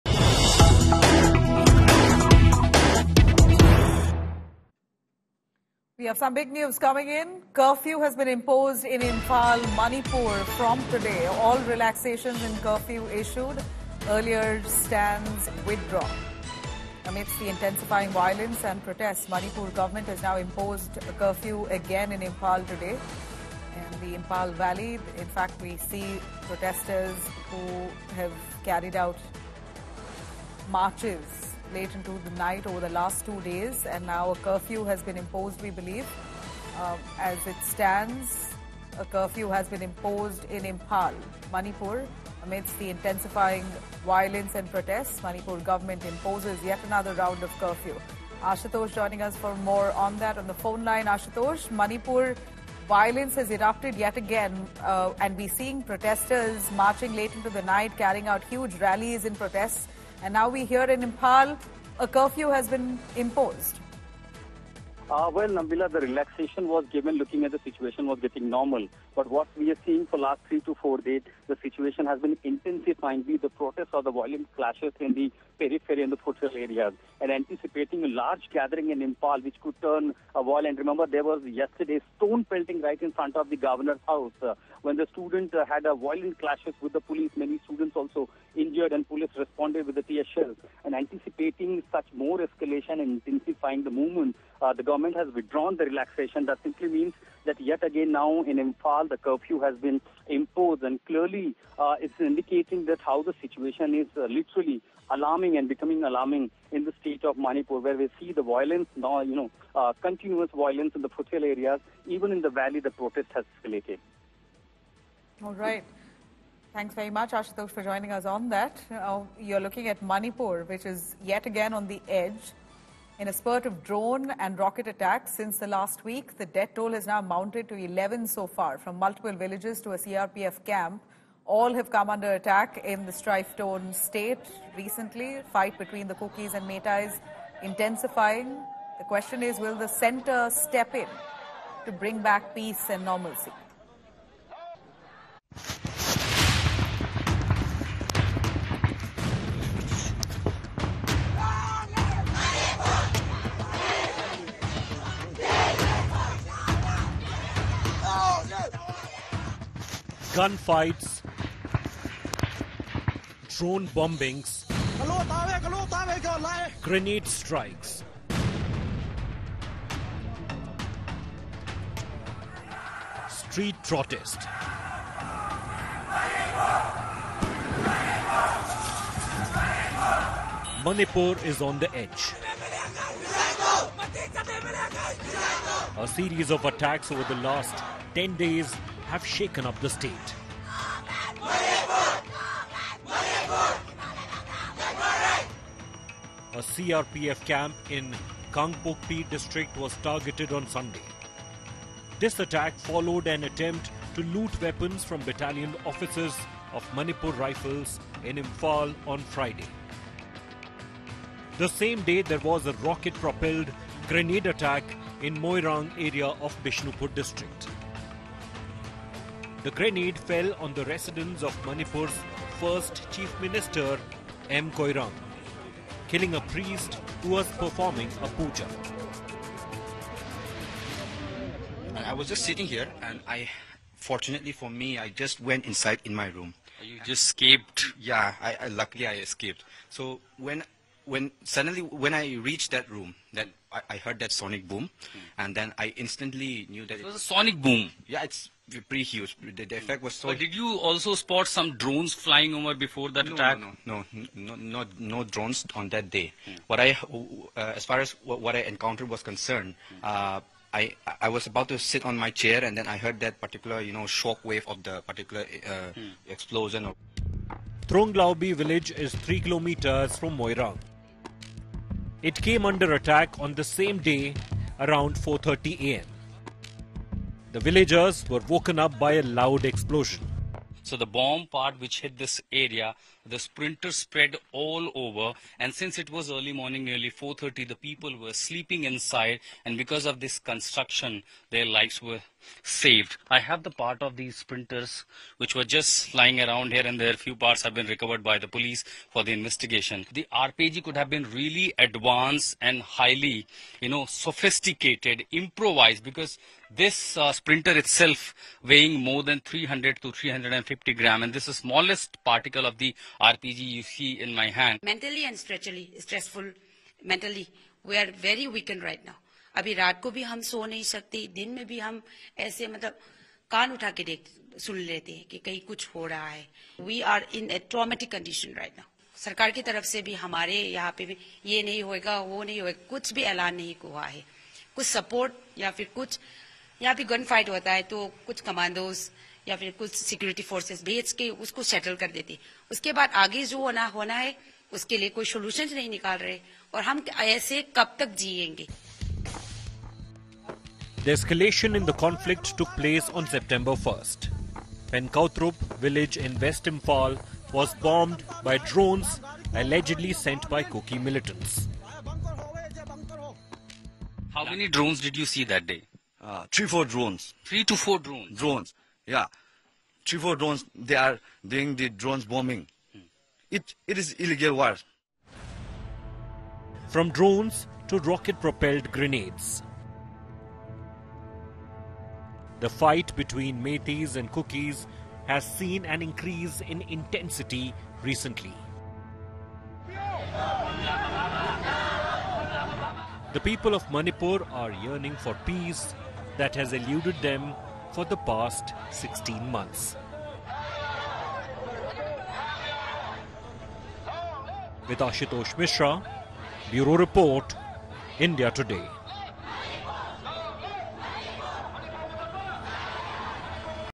Curfew Imposed In Three Manipur Districts Amid Drone, Rocket Attacks | India Today
In response to escalating violence and protests,A curfew has been imposed in three districts in Manipur on Tuesday amid the ongoing conflict in the state.The conflict in Manipur has escalated again, with recent incidents of violence and protests adding to the turmoil. Due to the escalation of violence, a curfew has been imposed in Imphal East, Imphal West and Thoubal districts.